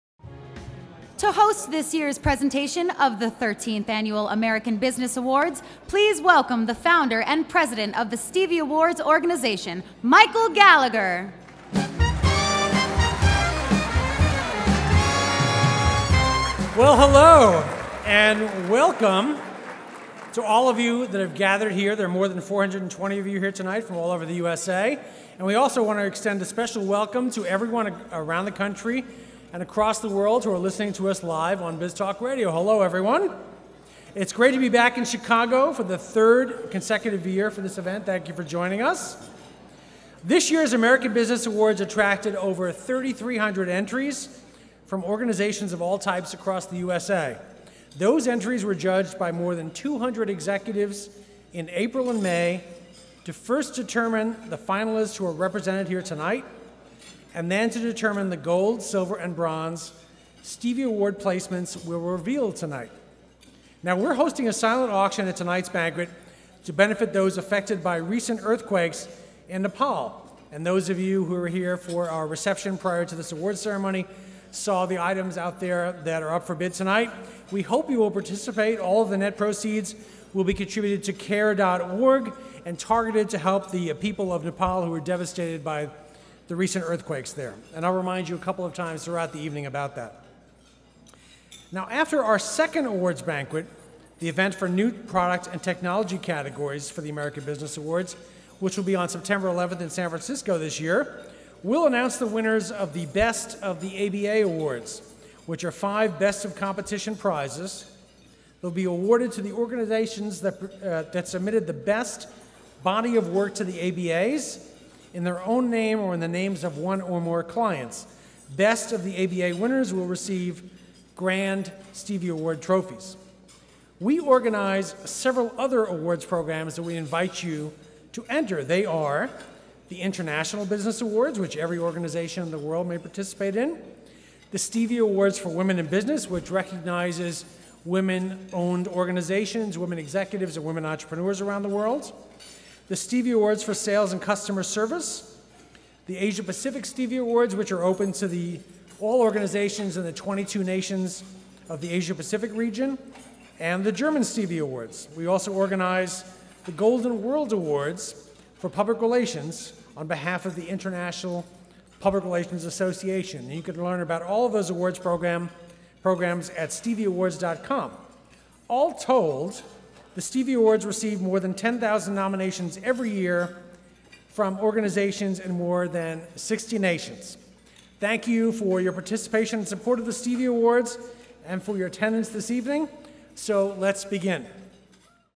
Opening Remarks at the 2015 Stevie Awards
The first group of Stevie Award winners in The 2015 (13th Annual) American Business Awards, the U.S.A.'s premier business awards program, were announced and presented on Monday, June 22nd at the Fairmont Millennium Park Hotel in Chicago, Illinois. The second group of winners will be announced at a ceremony at the Julia Morgan Ballroom in San Francisco on September 11.